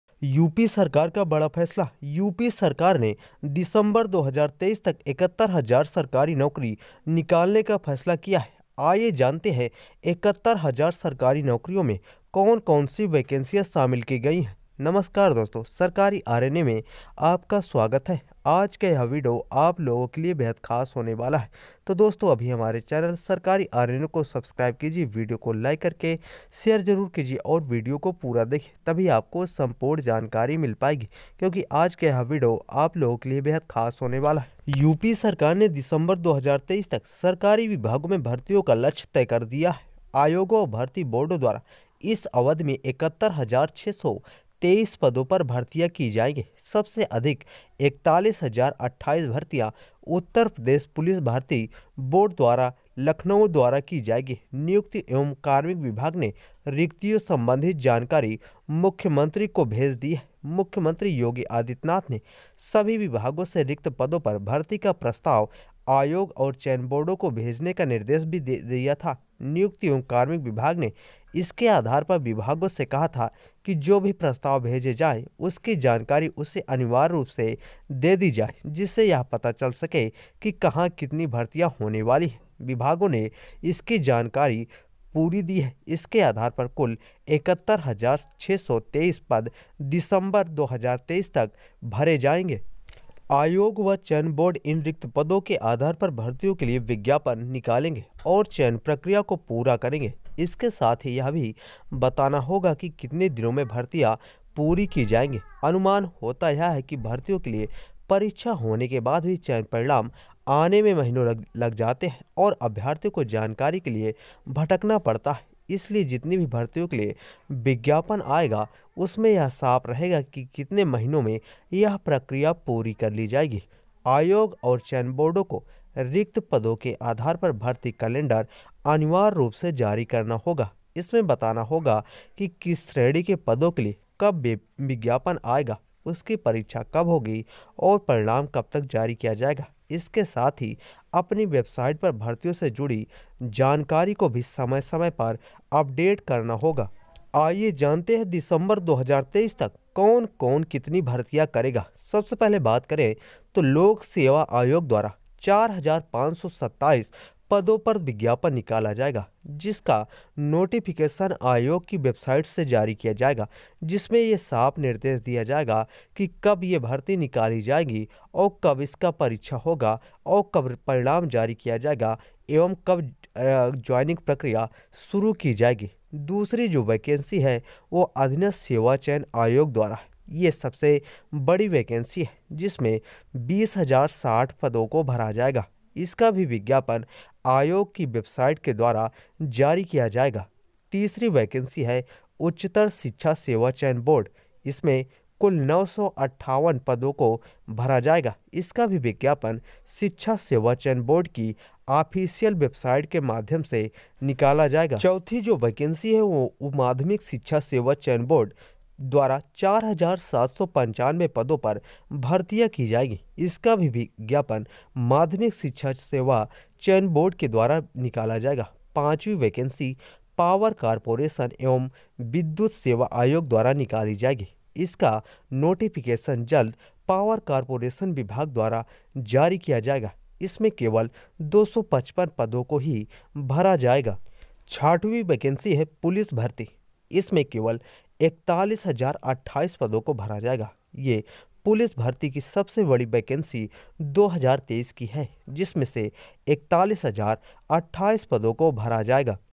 Cm Yogi का बड़ा फैसला, यूपी में होगी 71 हजार पदों पर भर्तियां,upsssc,up police, teacher के पद
यूपी में बंपर भर्तियां, दिसंबर 2023 तक 71 हजार सरकारी नौकरियां
यूपी की योगी सरकार दिसंबर 2023 तक सरकारी विभागों में भर्तियों का लक्ष्य तय कर दिया है। आयोगों और भर्ती बोर्डों द्वारा इस अवधि में 71623 पदों पर भर्तियां की जाएंगी।
यूपी सरकार ने दिसंबर 2023 तक सरकारी विभागों में भर्तियों का लक्ष्य तय कर दिया है। आयोगों और भर्ती बोर्डों द्वारा इस अवधि में 71623 पदों पर भर्तियां की जाएंगी। सबसे अधिक 41028 भर्तियां उत्तर प्रदेश पुलिस भर्ती व प्रोन्नति बोर्ड लखनऊ द्वारा की जाएंगी। नियुक्ति एवं कार्मिक विभाग ने रिक्तियों संबंधी जानकारी मुख्यमंत्री को भेज दी